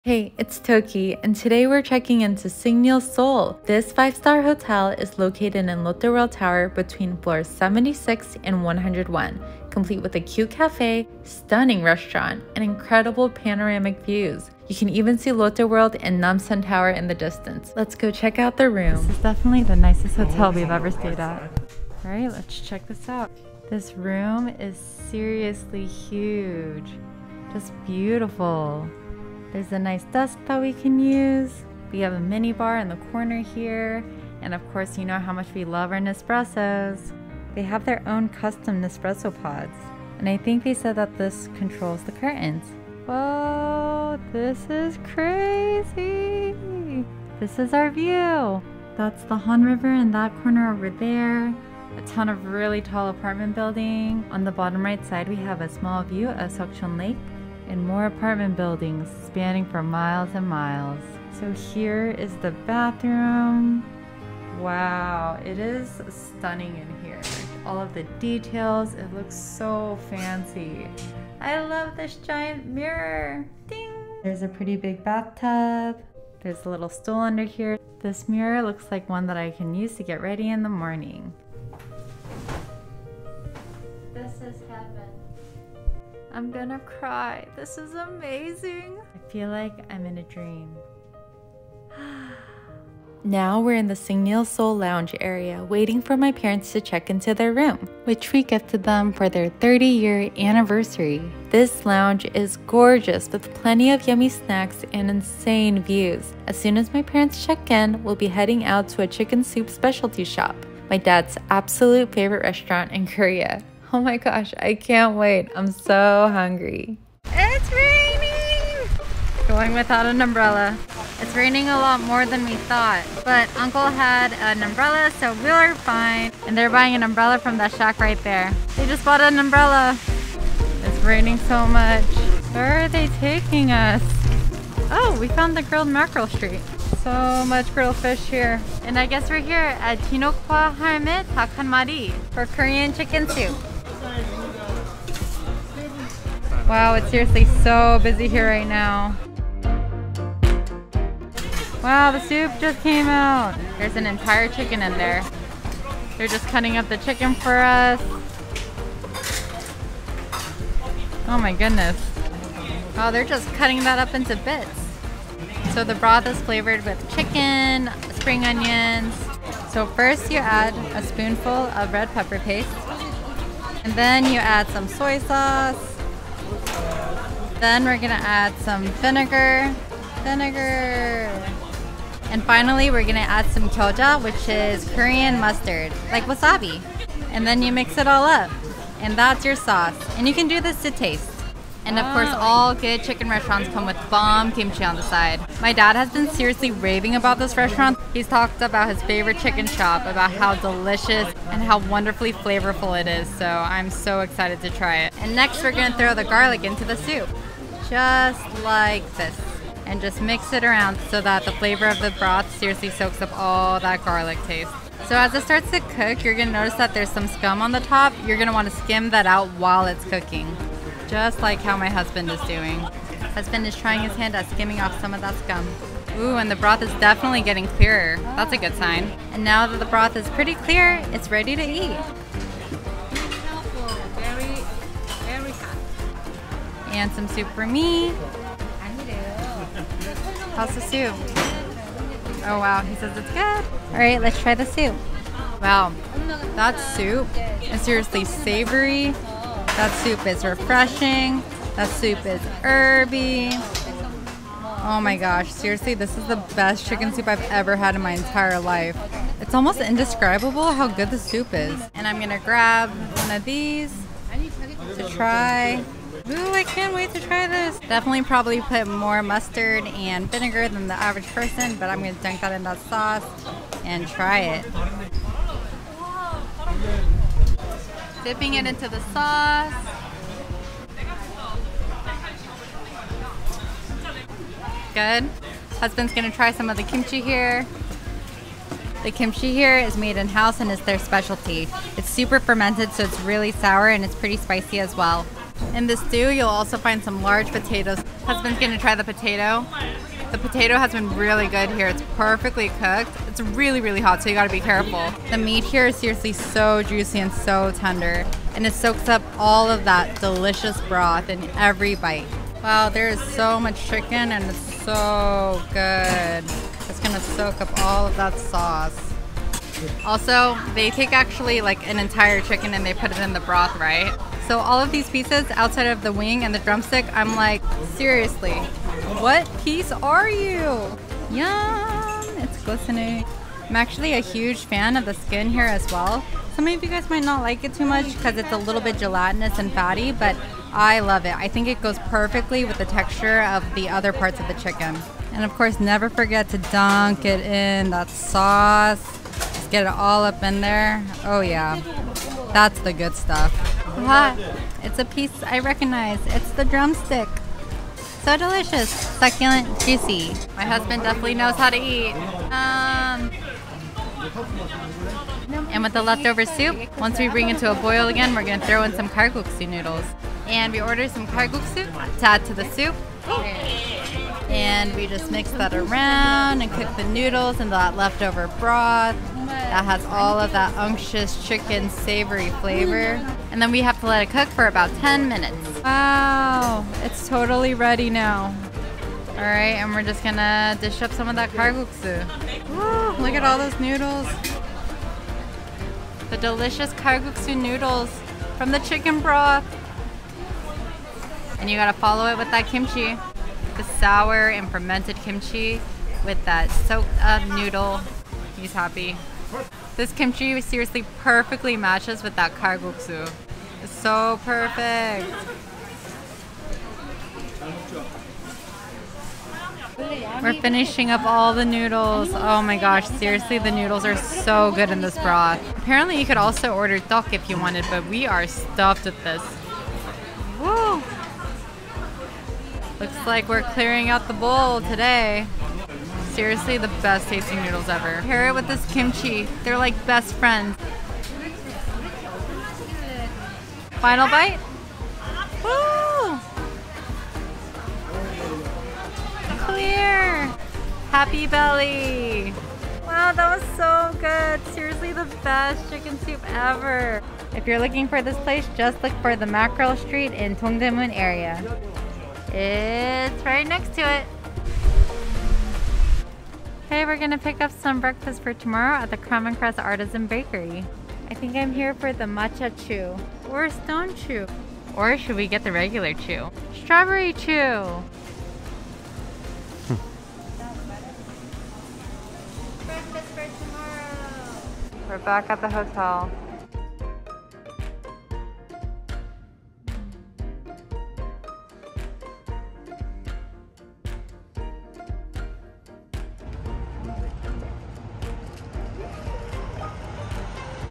Where to Stay in Korea | Signiel Seoul
Today, we're checking into Signiel Seoul Hotel! Our absolute favorite hotel in Korea! It is definitely on the pricier end, but if you're going to splurge - this is the hotel to pick! Highly recommend.

Also, we'll be dining at the best Korean chicken noodle soup restaurant ever! Complete with a full chicken cooked at your table with Kalguksu. Definitely add this restaurant to your list!

Details on the restaurant
진옥화 할매원조 닭한마리 본점
(진옥화할매원조닭한마리)
📍18 Jong-ro 40ga-gil, Jongno-gu, Seoul, Korea

With love,
TOKKI